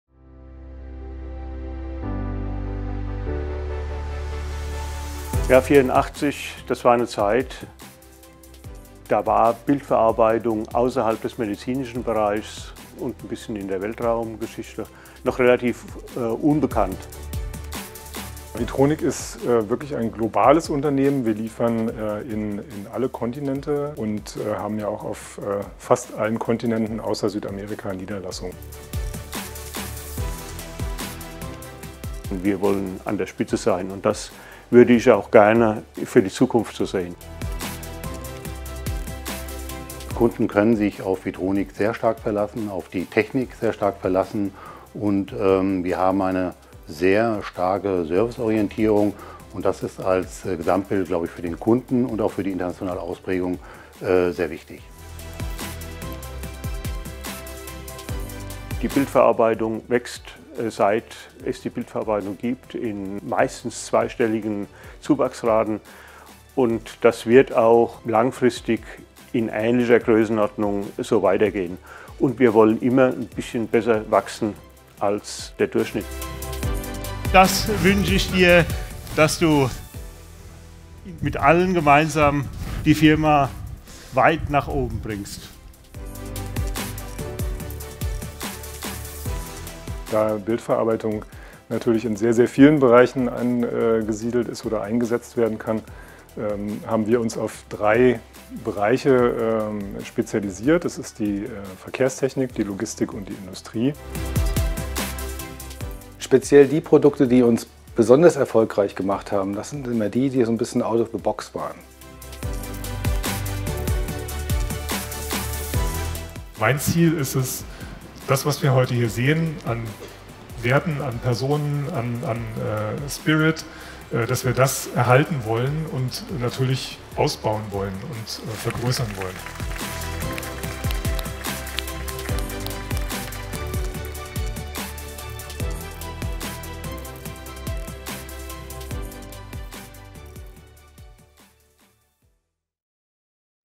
35th anniversary: Market Leader in Machine Vision | VITRONIC
VITRONIC was founded over 35 years ago and since then the company has undergone a remarkable expansion in machine vision technology. As one of the world's leading suppliers of machine vision in industries such as transportation and logistics automation, healthcare and automotive, VITRONIC has firmly established itself at the forefront of the market.

1984, the year VITRONIC was founded, was also the year of the blockbuster movie "Back to the Future". While Doc Brown and Marty McFly traveled through time in the film world, VITRONIC laid the foundations for a technological revolution. A young engineer in Wiesbaden founded a company that saw the world in a different light - without a time machine, but with machine vision systems. Dr. Norbert Stein recognized the potential of machine vision technology and began to develop innovative solutions for various industries.
Over the years, VITRONIC has achieved numerous milestones and brought innovative technologies to the market. The company has earned a reputation as a reliable and innovative partner with its products and solutions. From automated monitoring systems for road traffic to high-precision inspection systems for industrial production, VITRONIC offers customized solutions for its customers.

A key to VITRONIC's success is its focus on quality and precision and its family-run character, which continues to this day. Founded in Wiesbaden, Germany, the company continuously invests in research and development to integrate the latest technological advances into its products. By working closely with customers and partners, VITRONIC is able to offer innovative solutions that meet the needs of the industry.

VITRONIC's 35th anniversary marks an important milestone in the company's history. It is a dedication to continuous innovation and our commitment to consistently deliver outstanding quality so that you can trust us and our products. As part of the 35th anniversary in 2020, founder Dr. Norbert Stein took the opportunity to hand over his duties as Chief Executive Officer (CEO) of the global company to the next generation, Daniel Scholz-Stein.

📨 Sign up to our newsletter to get special news and insights about VITRONIC and our solutions

35th anniversary: Market Leader in Machine Vision | VITRONIC